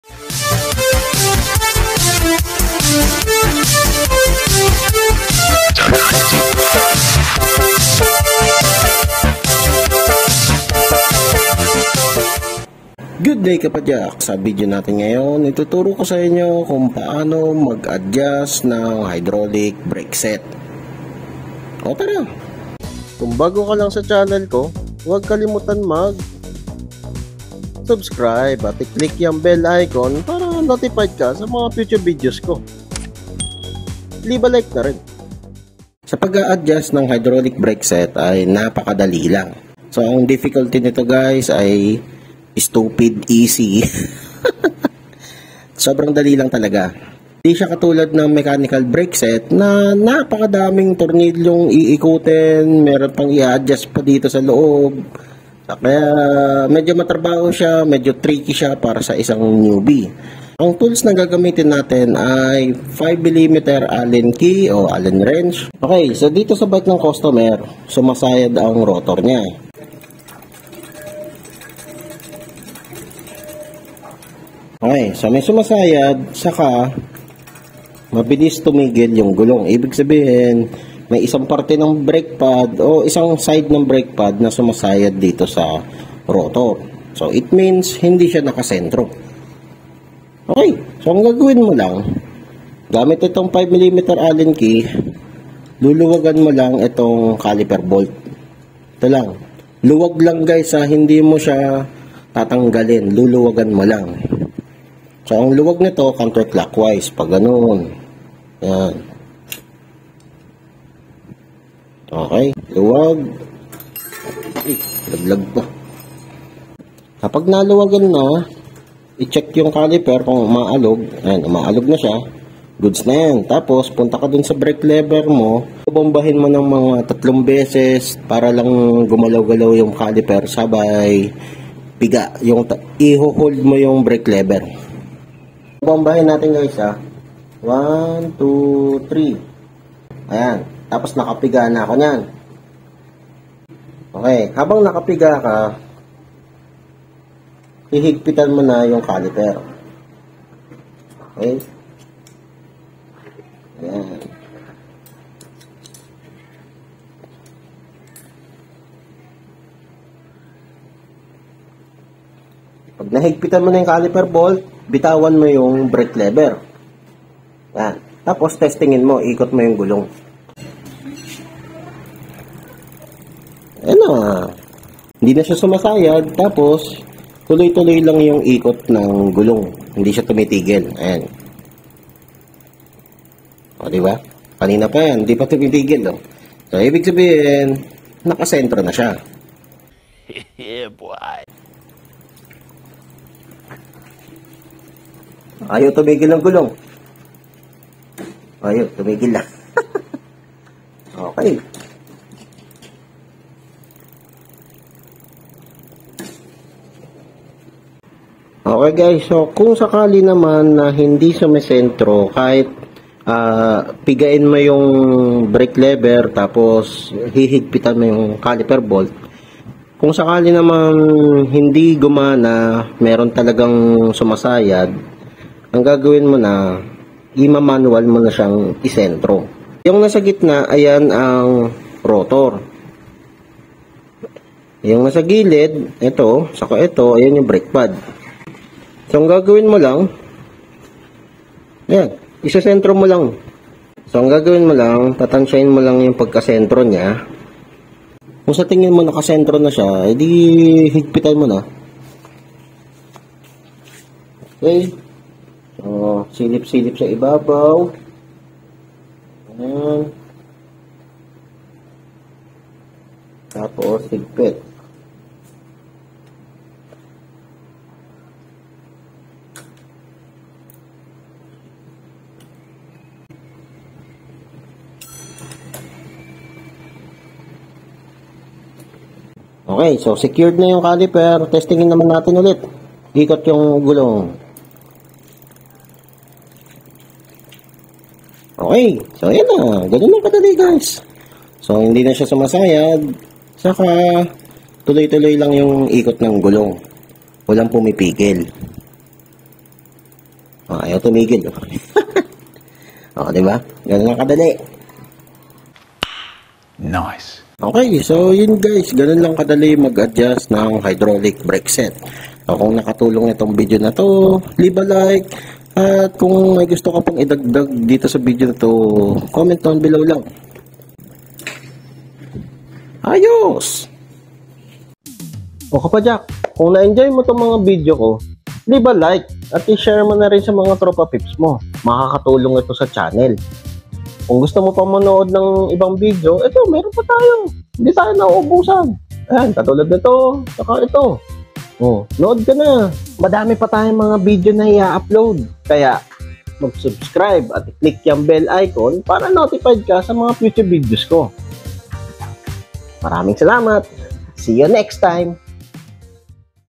Paano mag adjust ng hydraulic brake set | easy lang
Good day kapadyak sa video natin ngayon ituturo ko sainyo kung paano i adjust ang hydraulic brake set. Oh tara!